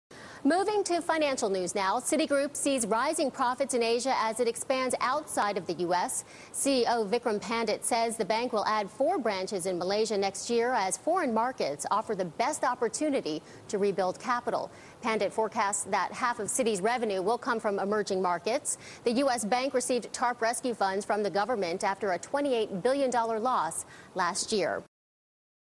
Citi Sees Rising Profit In Asia - Bloomberg
Citigroup sees rising profits in Asia. (Bloomberg News)